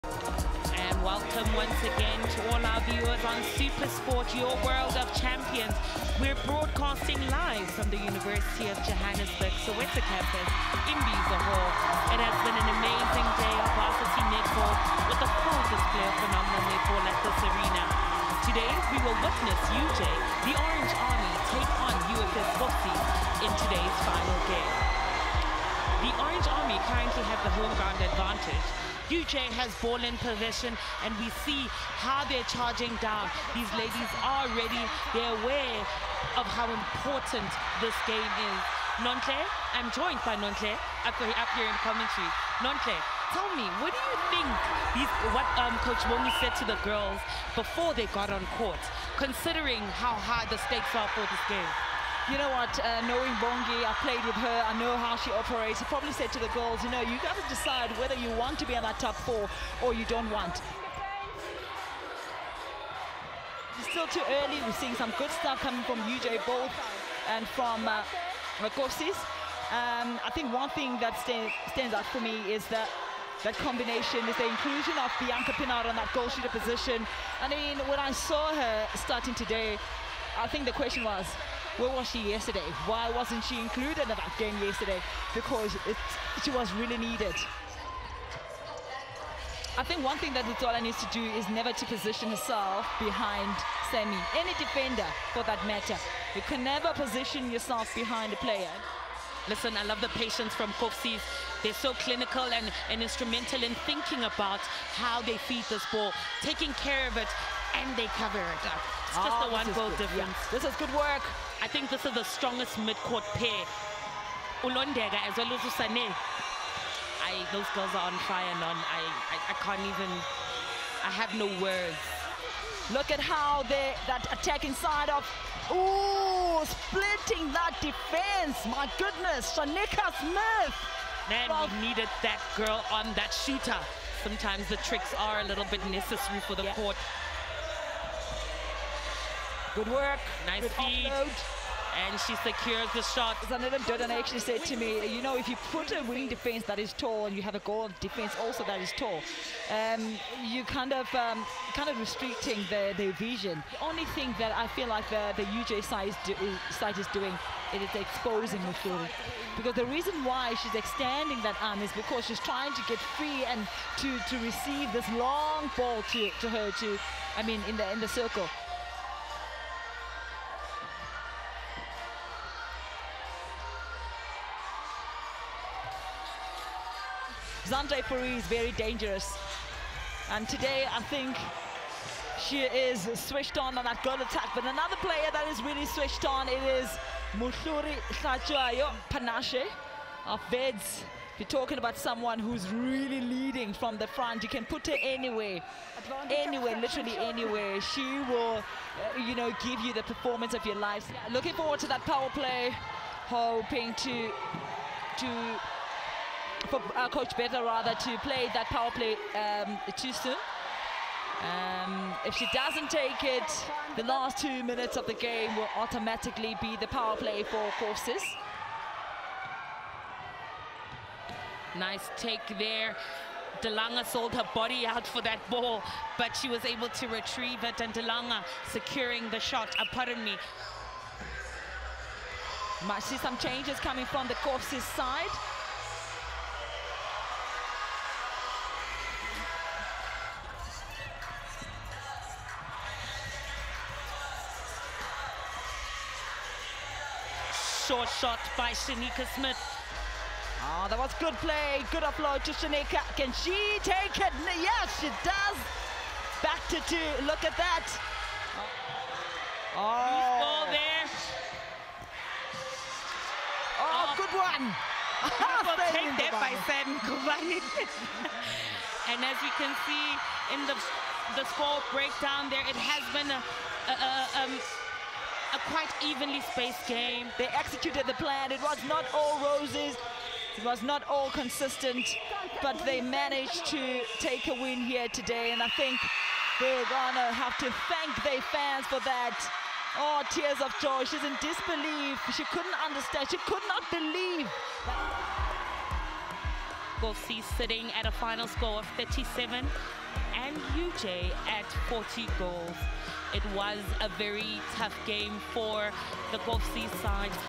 Round 4 HIGHLIGHTS: UFS vs UJ - 27 August
Watch as the UJ Orange Army stun four time defending champions Kovsies in round 4.